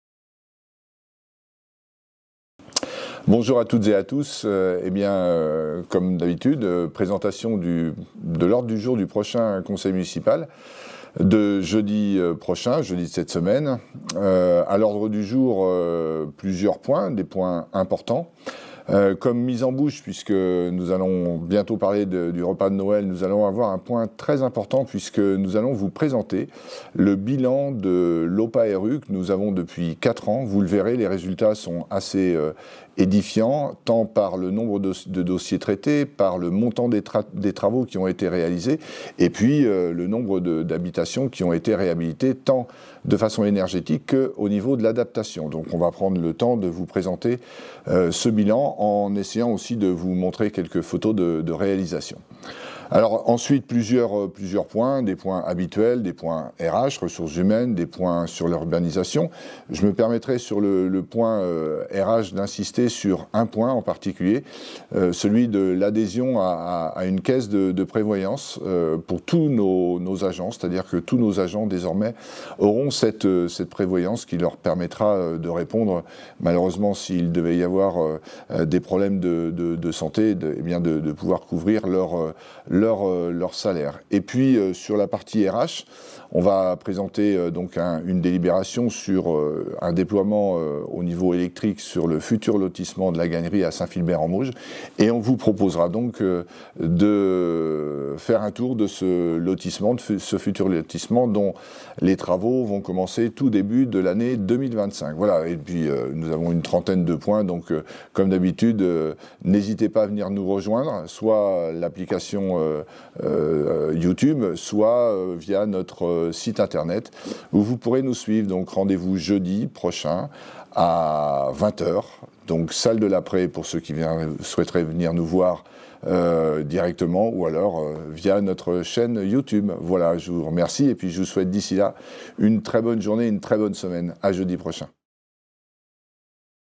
Conseil municipal : l'ordre du jour du 28 novembre 2024
Le prochain conseil municipal aura lieu jeudi 28 novembre à 20h à la salle de La Prée - Le Sporting, à Beaupréau.

Franck Aubin, maire de Beaupréau-en-Mauges, vous présente les sujets qui seront abordés !